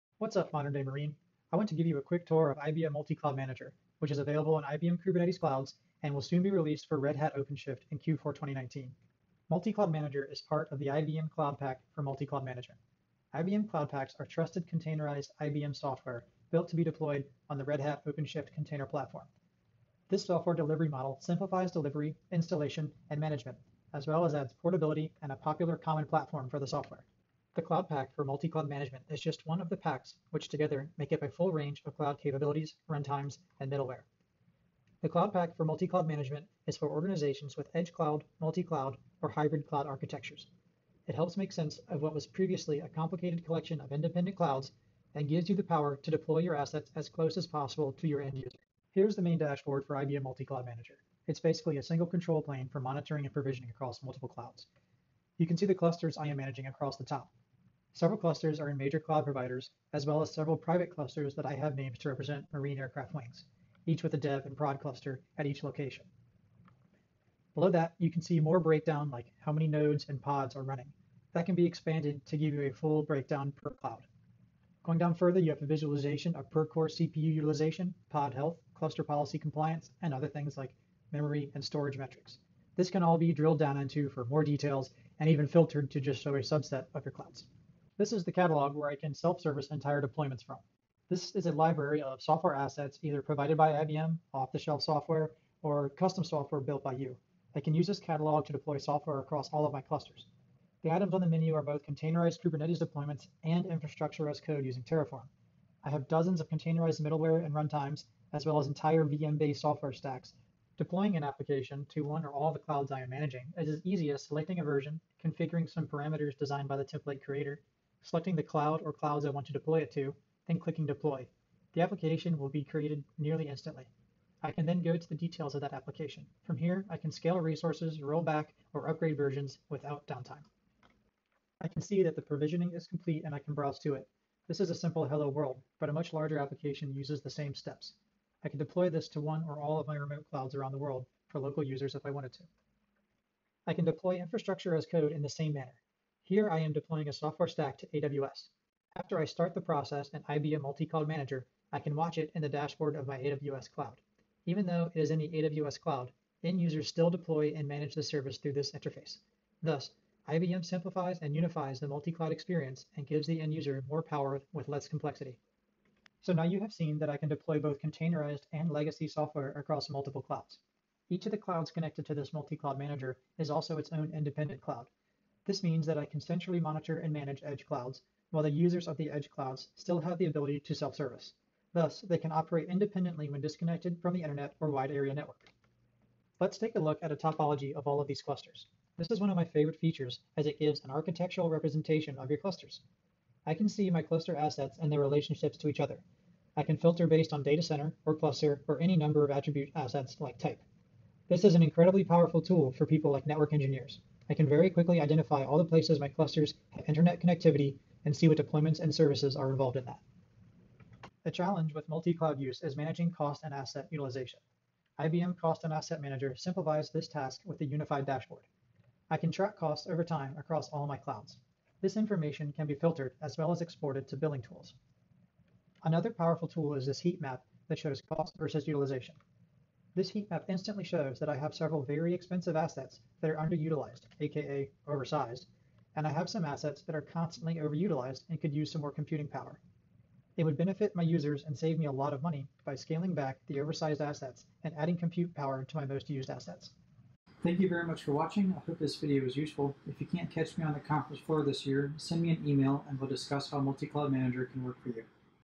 IBM Multi Cloud Manager for Modern Day Marine and AFCEA West
Made specifically to showcase USMC use cases, but applicable to any multi-cloud scenario, this video shows some of the ways the IBM Multi Cloud Manager can help you make sense of Day 2 operations in the cloud. Made from footage and environments from my colleagues at IBM, Keaton Willis and Jason Hamilton.